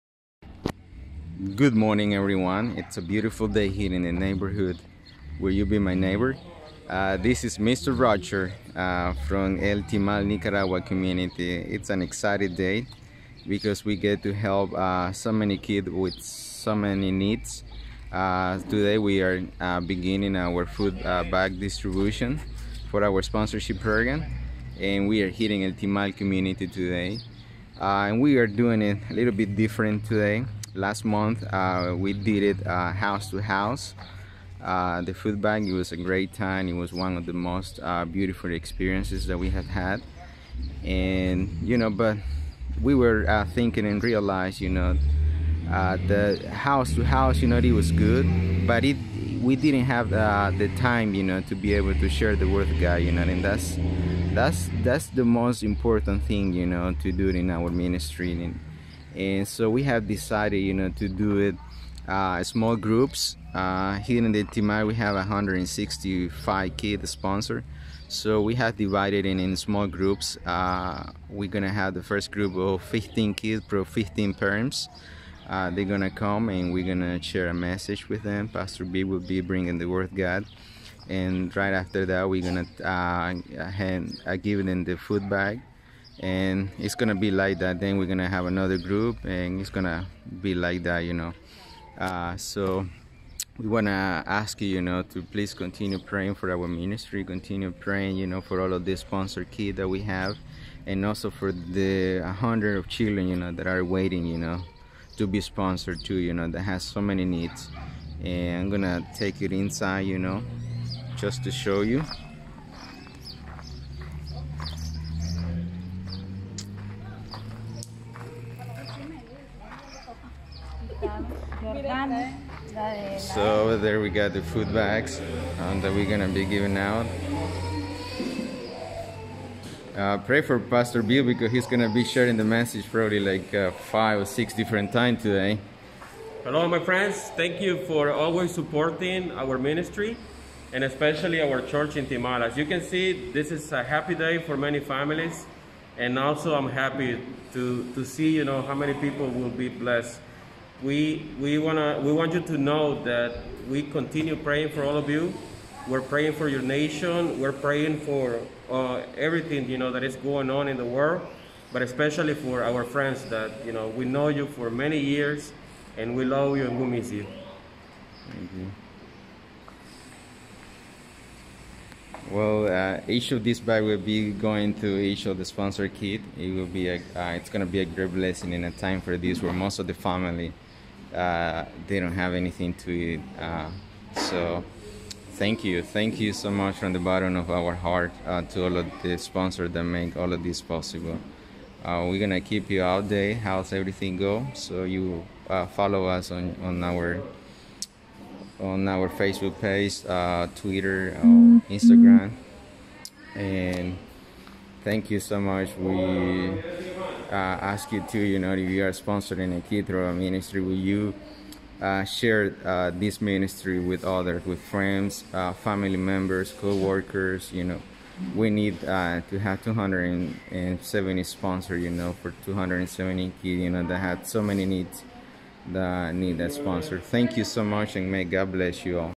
Mr Rogers in Nicaragua?
Will you be my neighbor? Mr. Rogers (AKA Roger Hurtado) gets ready for food bag distribution for more than 100 sponsored children in the El Timal Barrio.